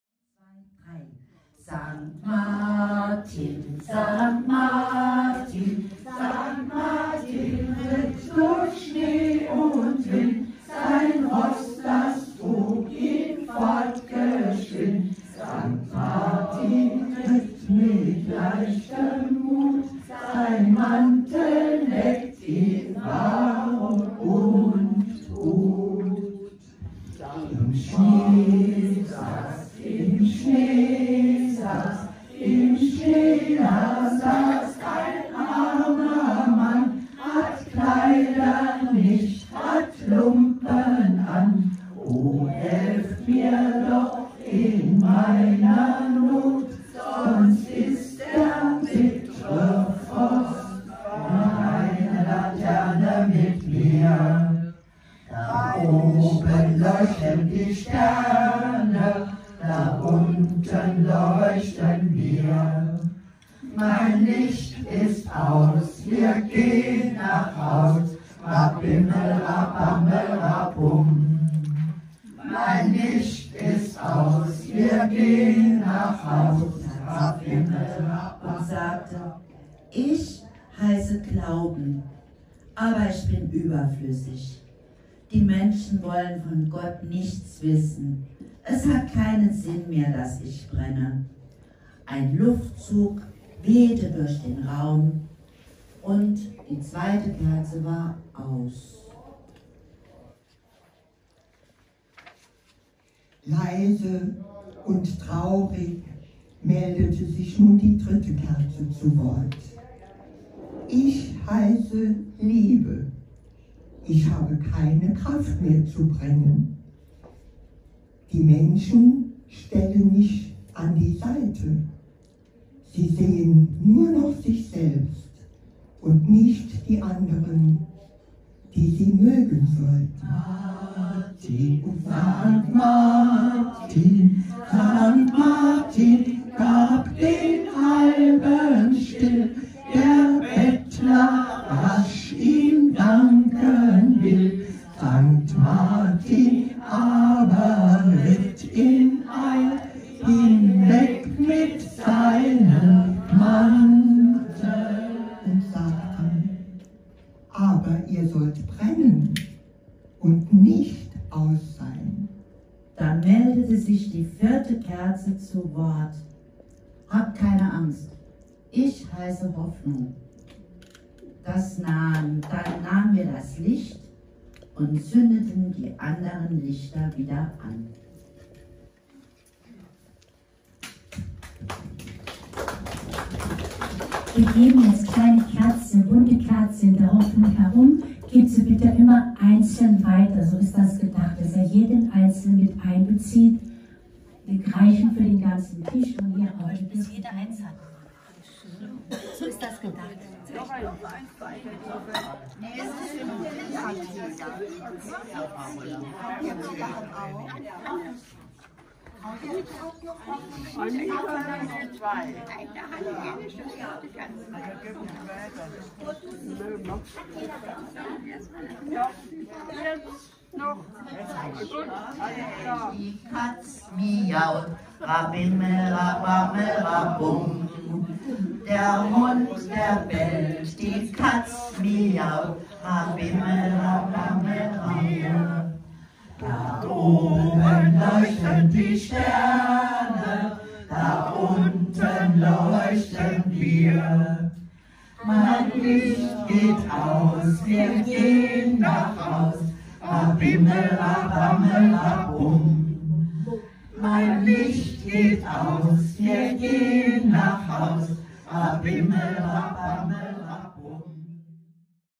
St Martins Feier 2023 bei der AWO Malstatt
Besinnliche Feier in der Begegnungsstätte des AWO-Ortsvereins  Malstatt am 8. November 2023 auf dem Saarbrücker Rastpfuhl